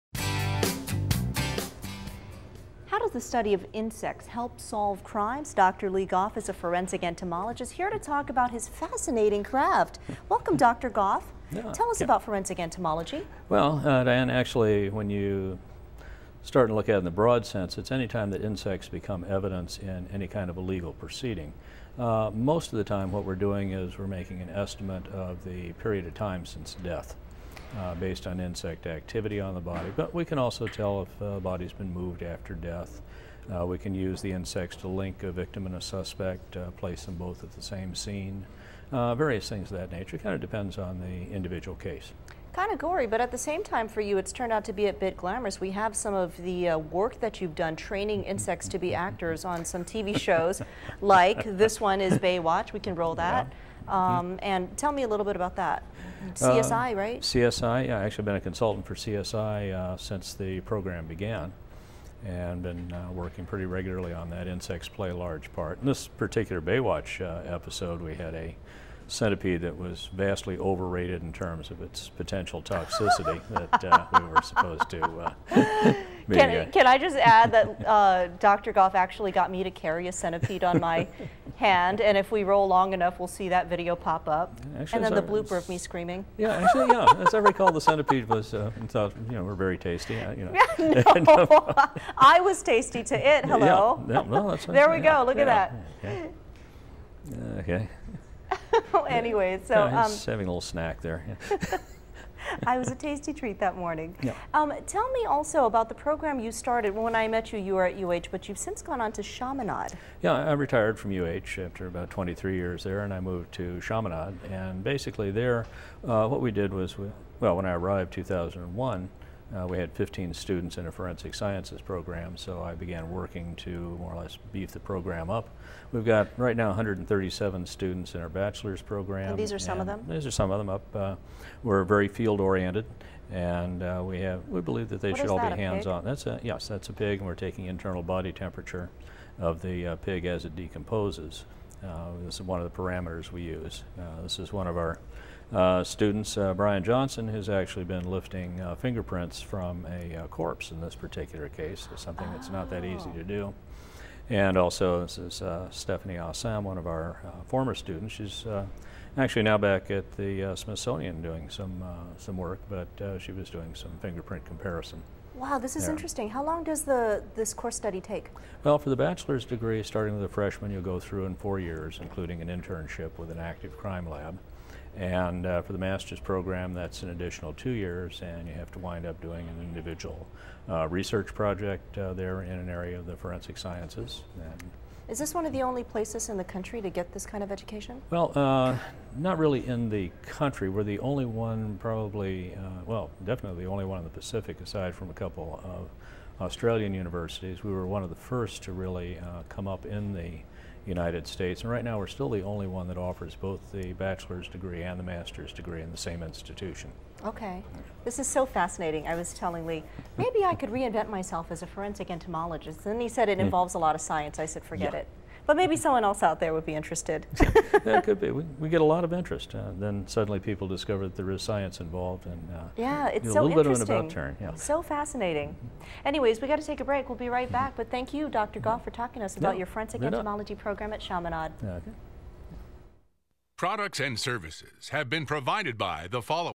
10/15/09 KHNL- Forensic entomologist Dr. Lee Goff
How does a maggot help reveal the time a corpse died? National pioneer in the field of forensic entomology Dr. Lee Goff visits the KHNL morning show to talk about his popular Chaminade University program.